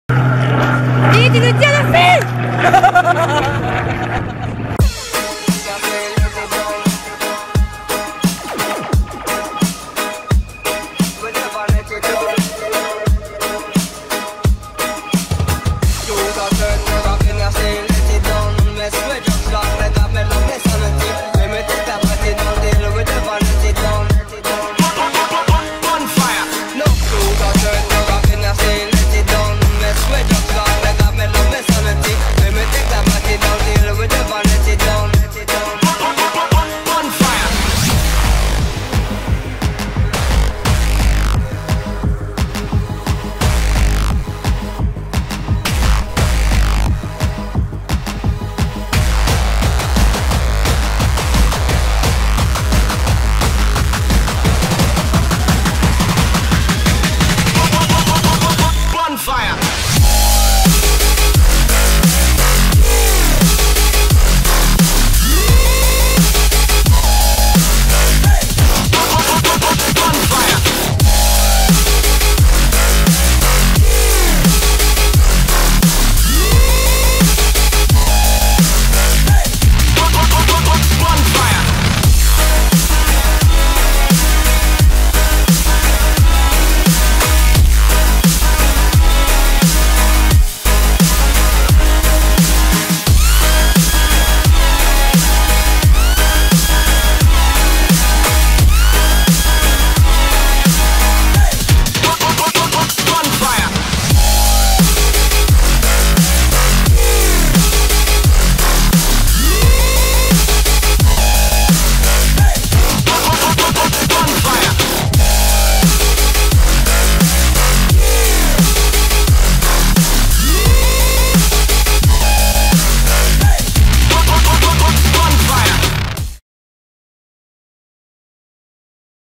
Paye ton Juin !!
Conneries et ski à Avoriaz au mois de Juin à regarder en HD